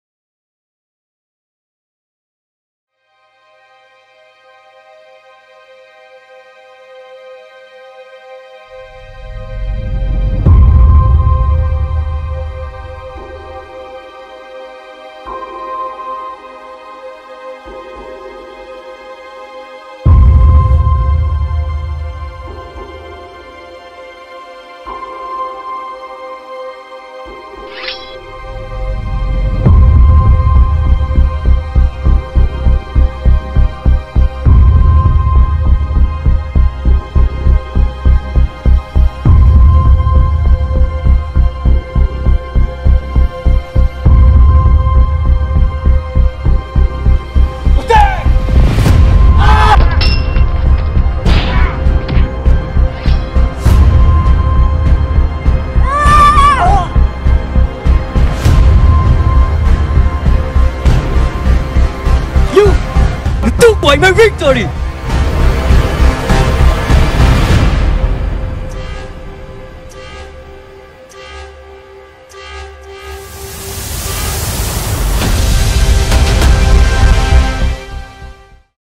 MULAN DRAMA TRAILER | 27TH AEC GALA RECOGNITION 2018 OFFICIAL DRAMA TRAILER
This is a story from China where women should only get married and have children. When China was attacked by the Hun armies, one man of every family should go to war. Mulan didn't want her father to go and she disguised herself to become a male warrior. She did it because she wanted to protect her father  so that her father needn't to go to war. it was also because of her love to the kingdom. Could Wulan undergo her plan... or just surrender her fate as an ordinary woman?
Edited & Modified by Sri Seti Indriani
Directed by Ayu Luthfia, Anggun N. Efriena
Trailer Produced by Ayu Luthfia, Anggun N. Efriena, Armia Tri Akbarwati, Ecky Alimansyah & AEC Documentation Team